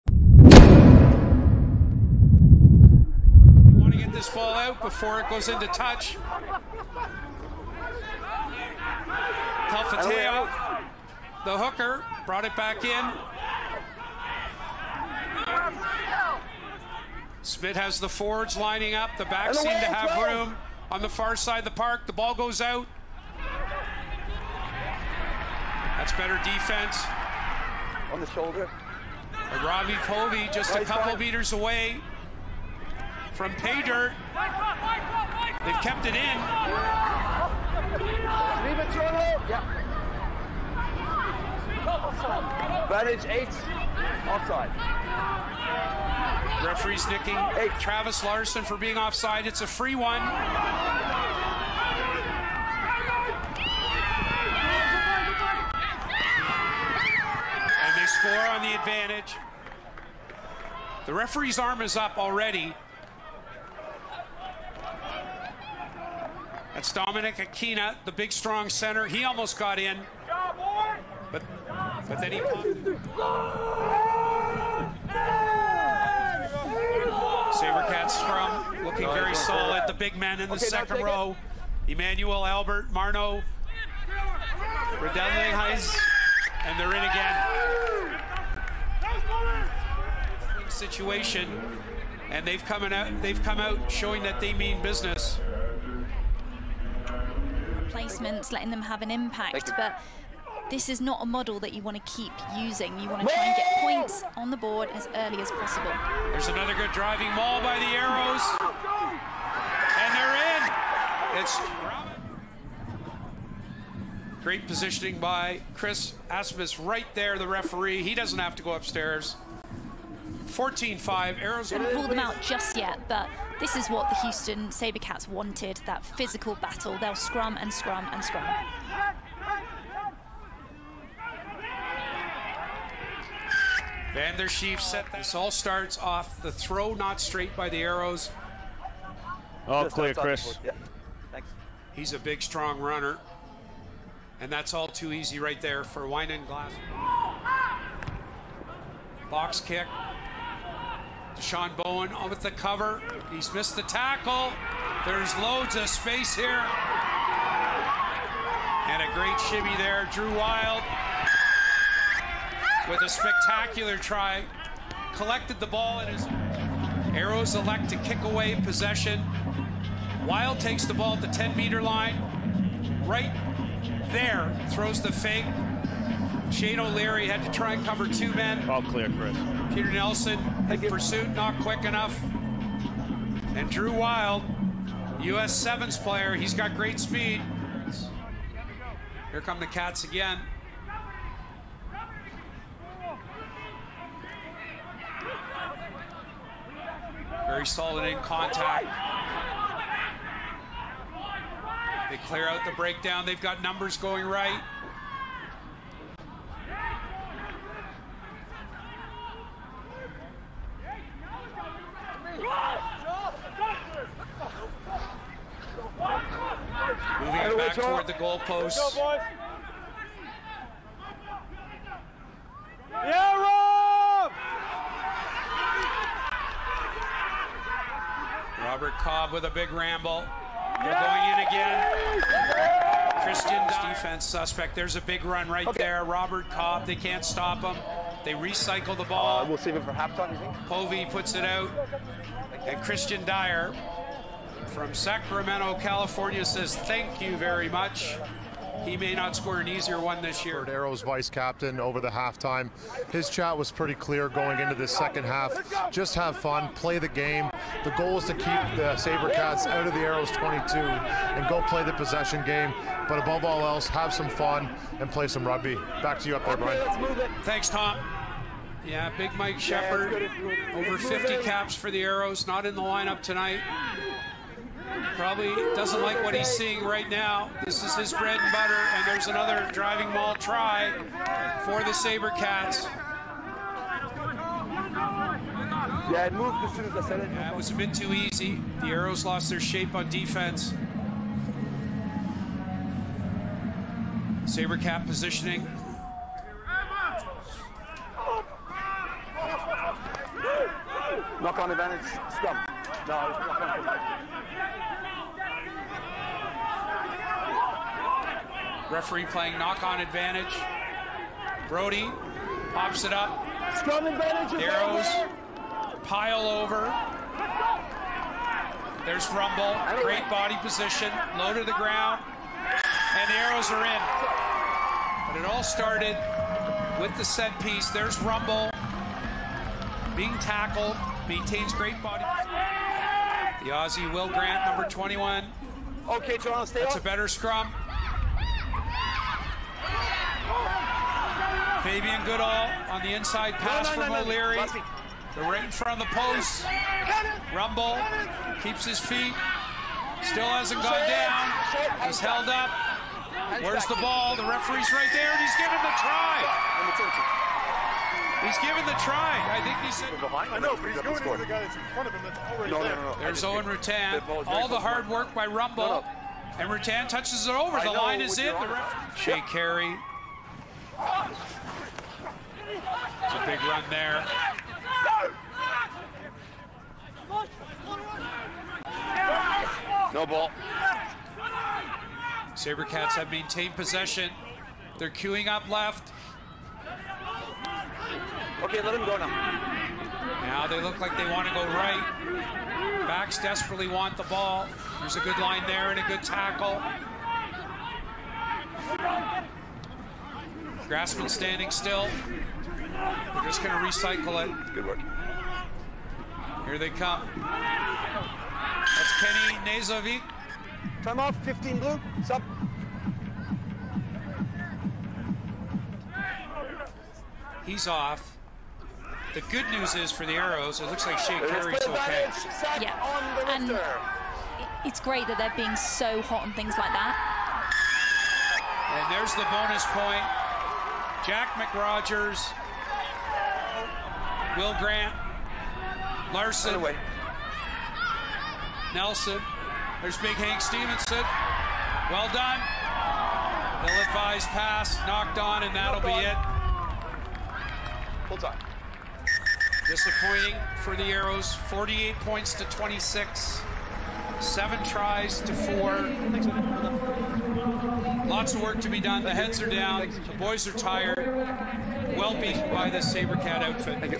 HIGHLIGHTS | Toronto vs Houston
The Toronto Arrows host the Houston SaberCats in Week 16 of MLR's 2023 season.

For more information about MLR, go to the league's official